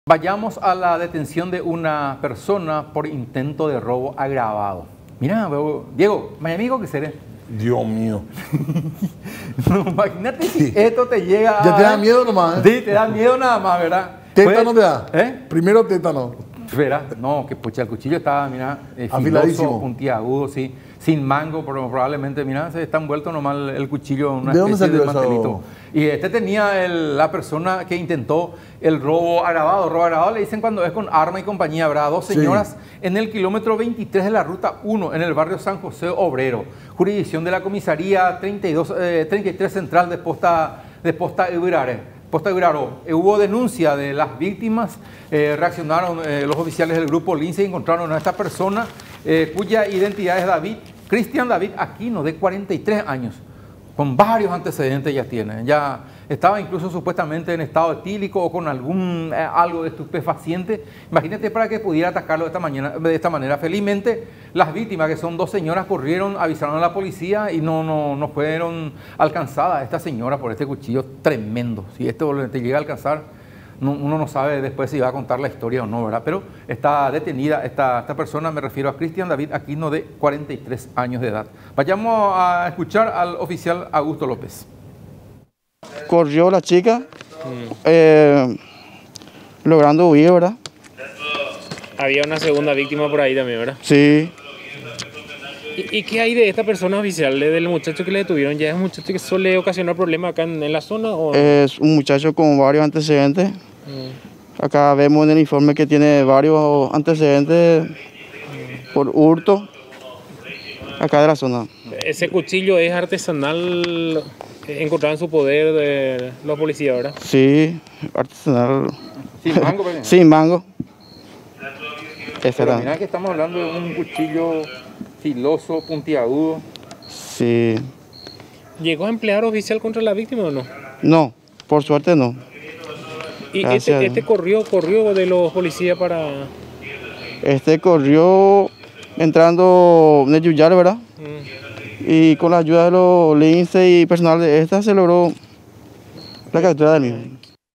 Detienen a un hombre por intento de robo
En el kilómetro 23 de la ruta 1 en el barrio San José Obrero fue detenido Cristian David Aquino de 43 años y con varios antecedentes, por intento de robo agravado.